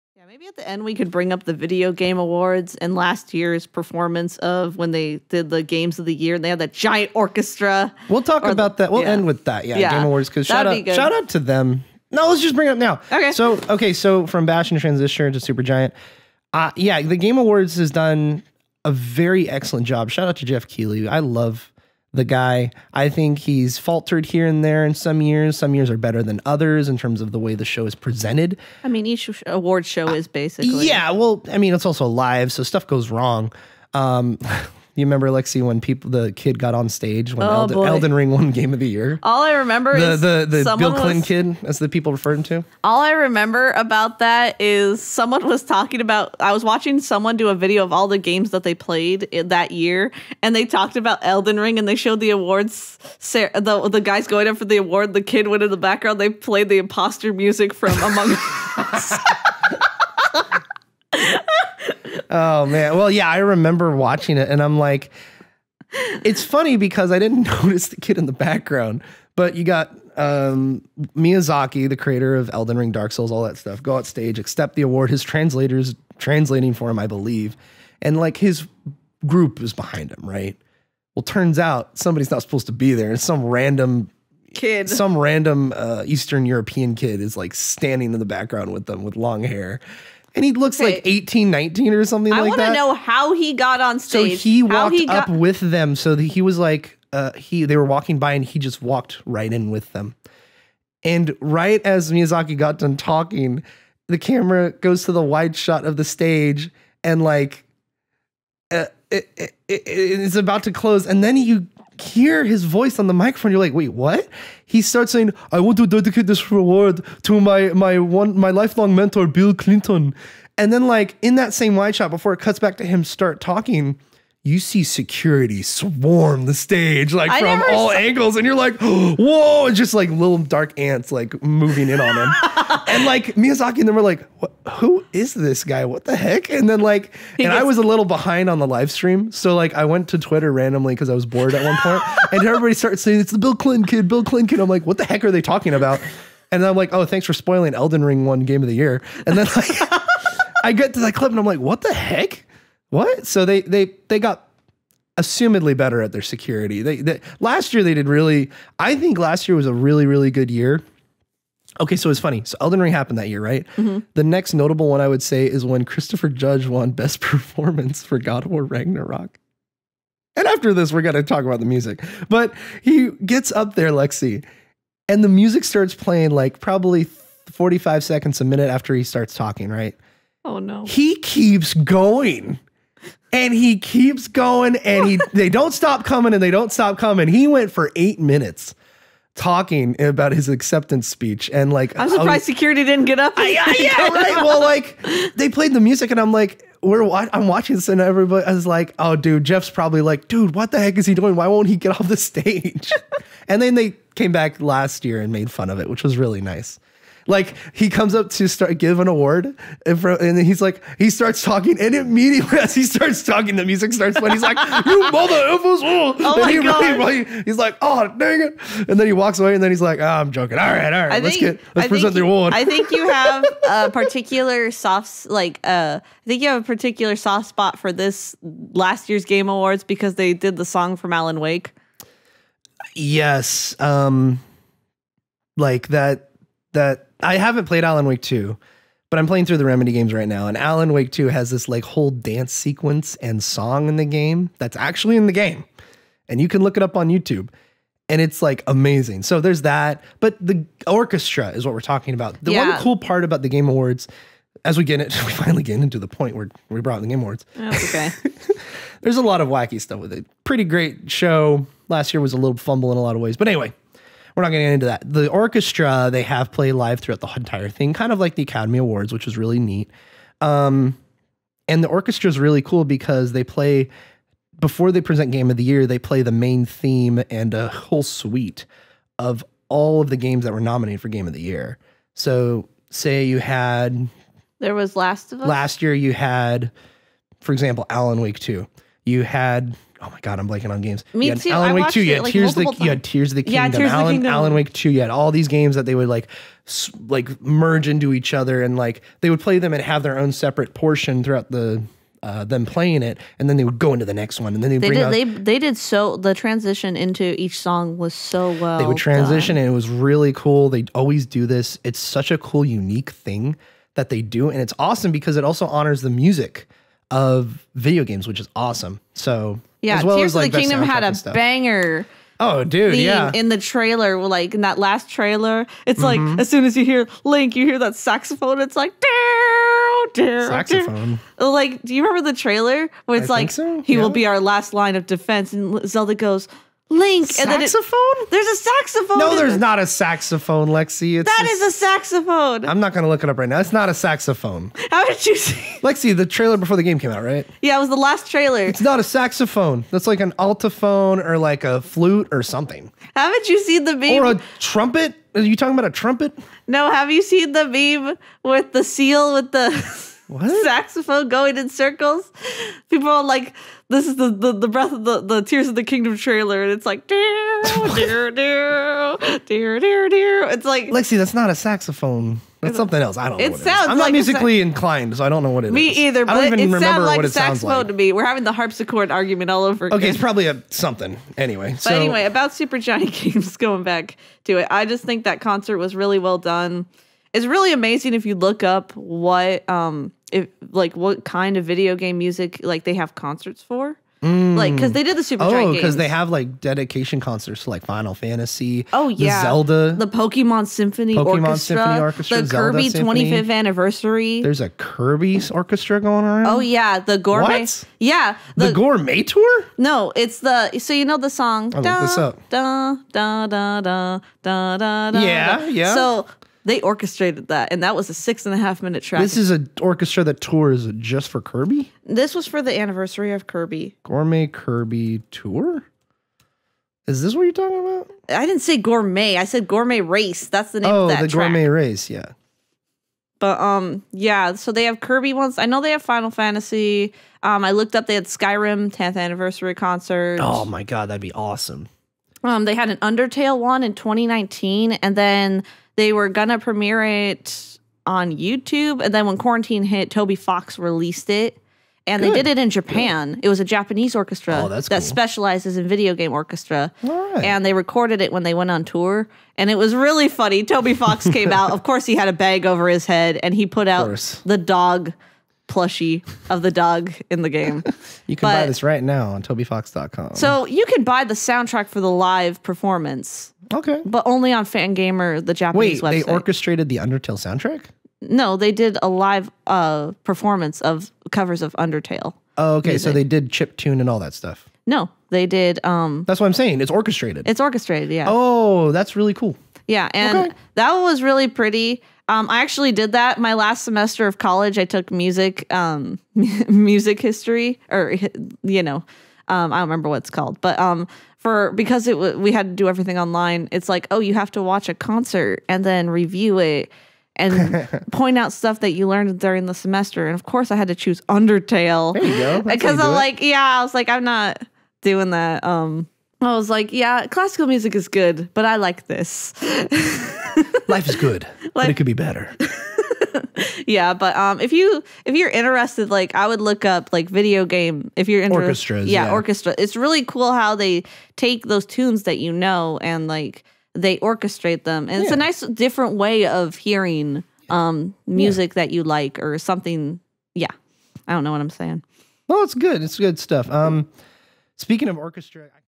The Game Awards and Video Game Orchestras | A Snarp Fangle Podcast Highlight
That kid was in BIIIIIG trouble!